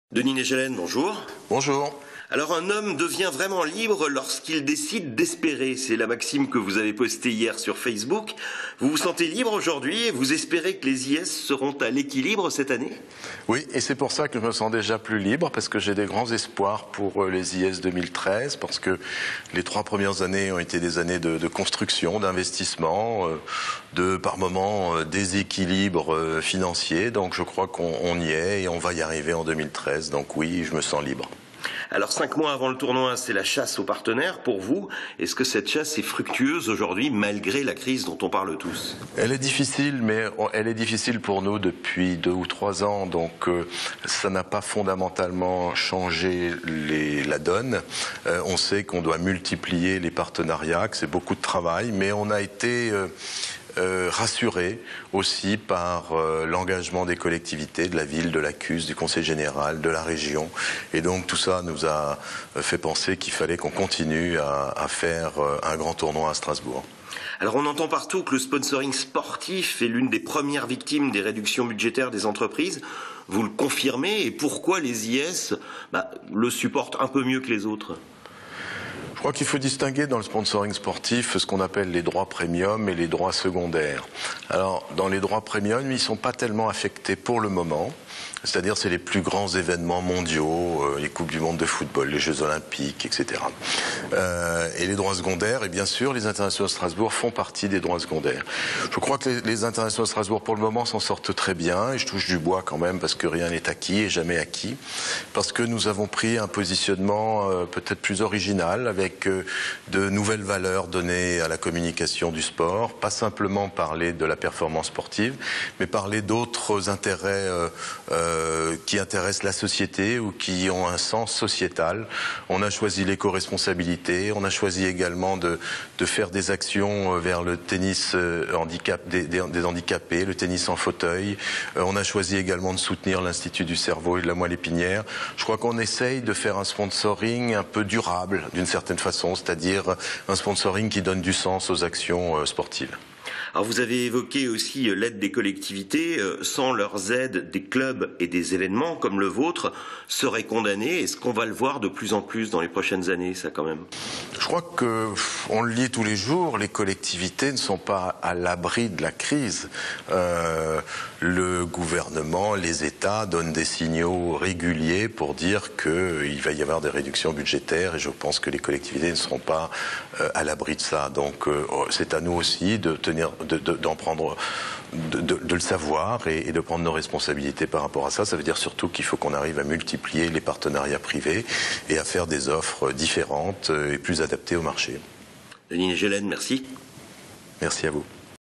Les IS à l'équilibre en 2013? (Alsace)
Sites webs/blogs, copiez cette vidéo avec le bouton "Intégrer" !
 Entretien avec Denis Naegelen, directeur des Internationaux de Strasbourg.